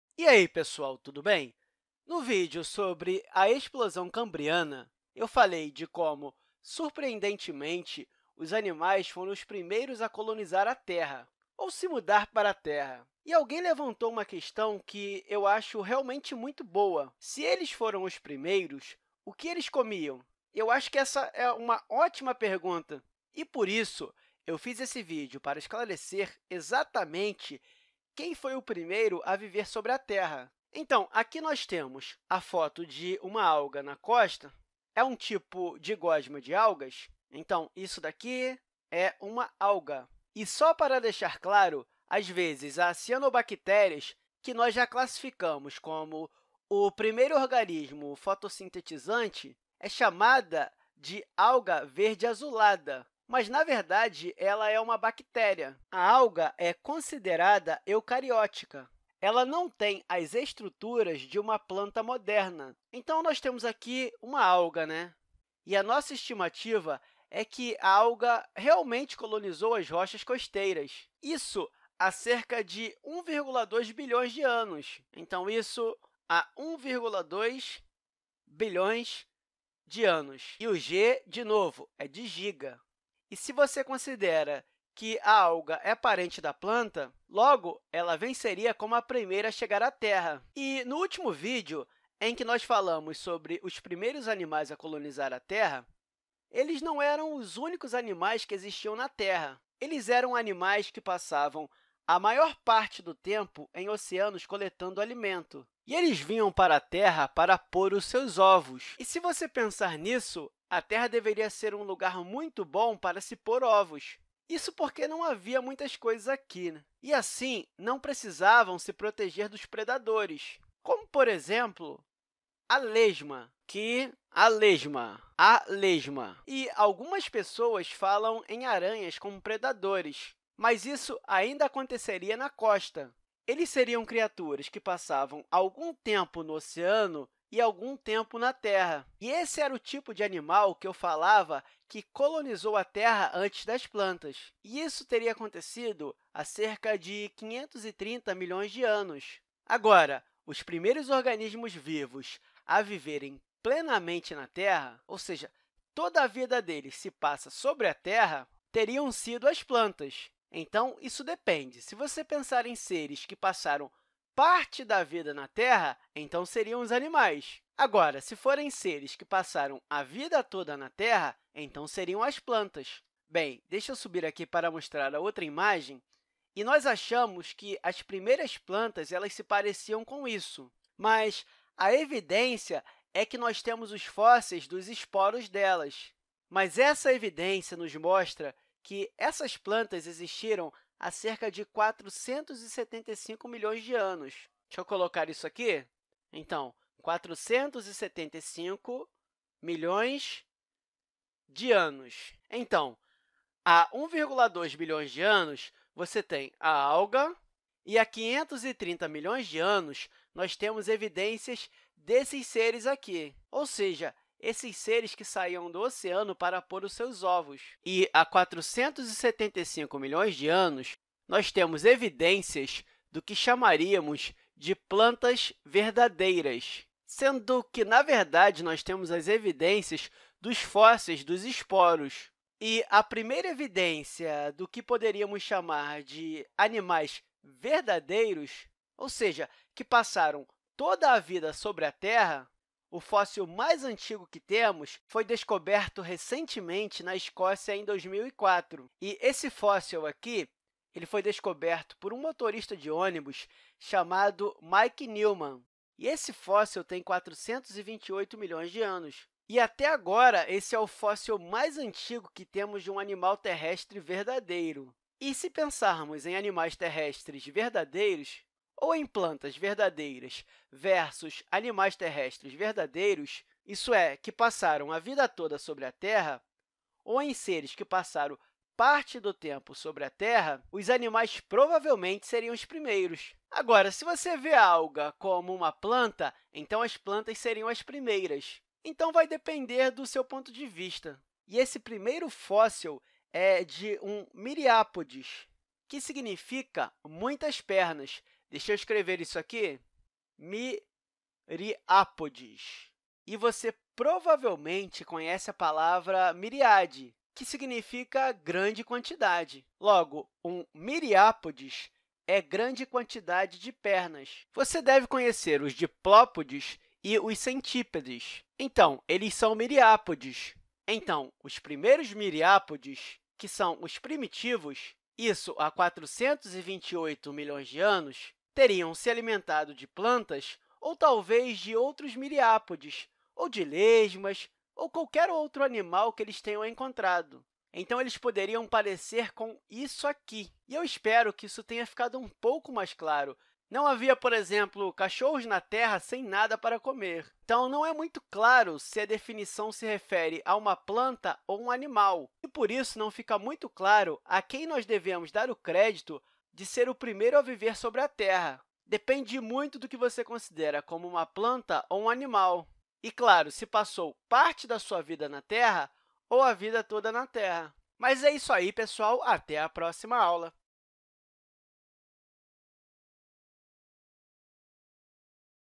Primeiras coisas vivas no esclarecimento da terra | Terra e universo | Khan Academy
Este vídeo ensina: Primeiras coisas vivas sobre a clarificação da terra (Este vídeo tem direitos autorais sob a licença Criar Atribuição e Compartilhamento da mesma licença CC-BY-SA). Criado por Sal Khan .

Perdeu a aula anterior?

Física da Ciência Cosmologia e astronomia Escala do universo Big bang e expansão do universo na Khan Academy: Nesse vídeo nós mostramos como surgem as primeiras formas de vida na Terra.

Quer um tutor virtual para Terra e universo?  Habilite a missão Terra e universo na Khan Academy